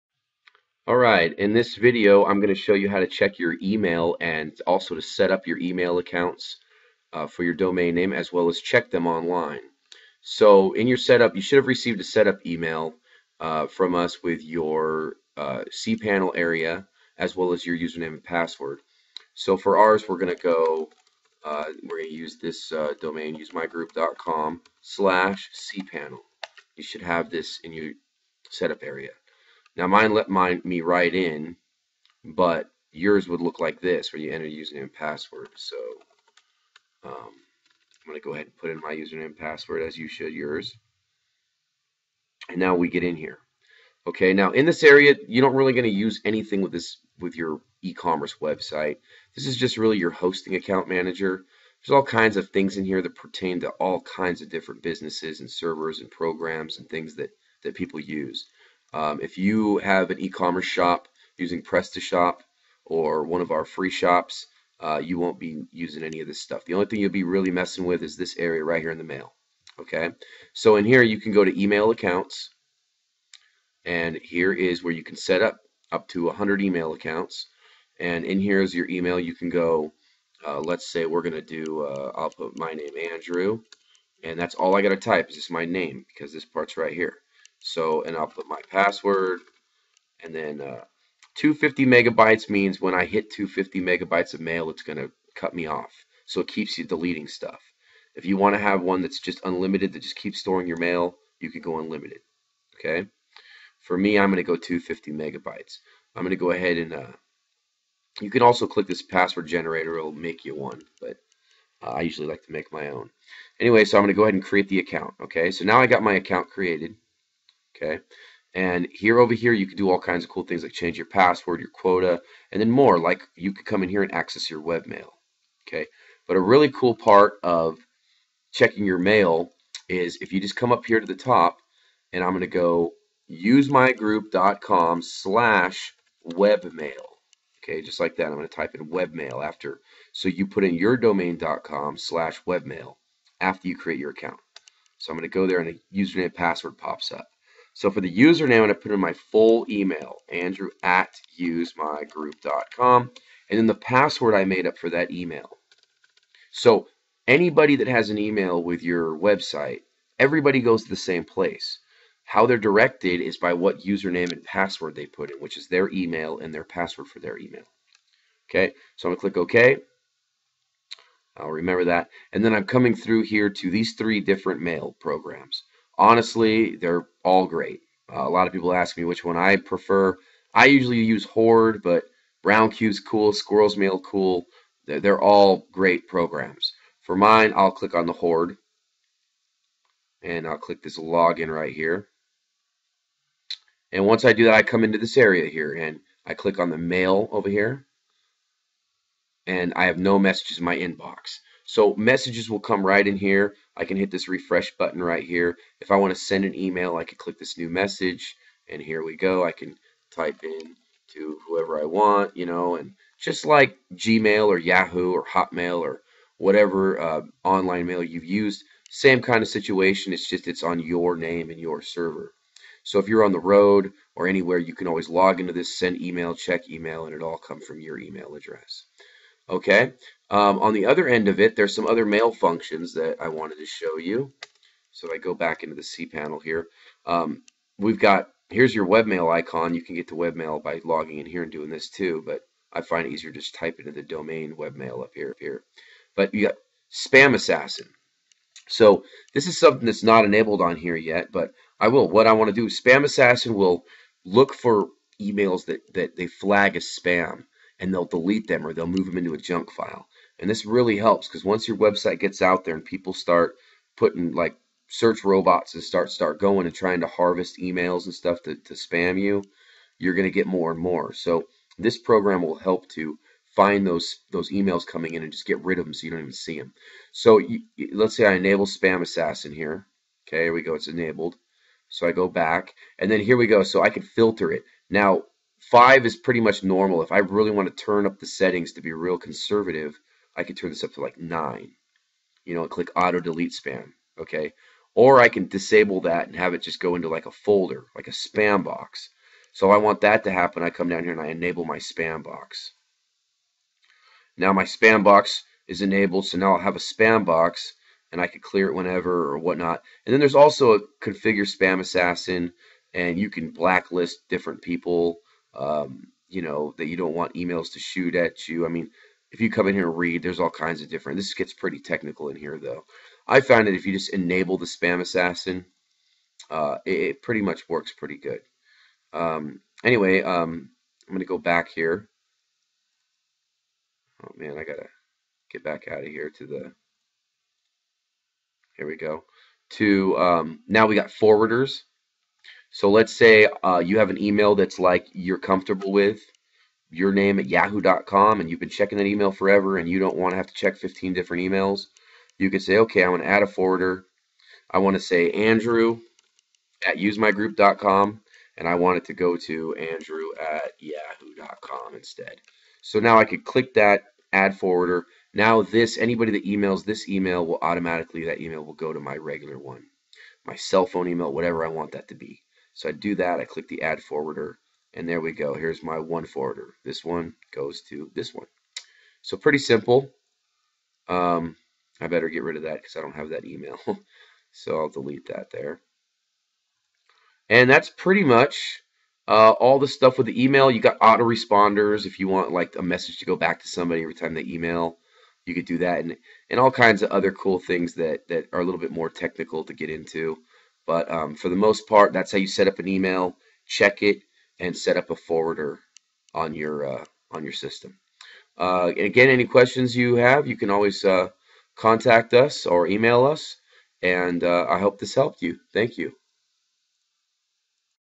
Cpanel Tutorials | Setting Up Email Addresses | Video Training
Setting up your email addresses using the cpanel service.